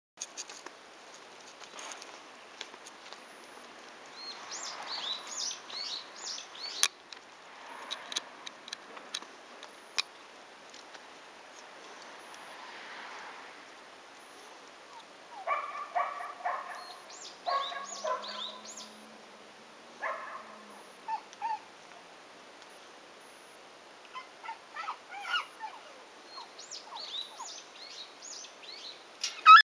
柴犬の仔犬 生後111日目その１
初めてのお散歩です。この仔犬は首輪をすごく嫌がるので、ハーネスにしました。U字溝なんかも平気で超えますけど、柵に引っ掛かってキャンキャン鳴きました。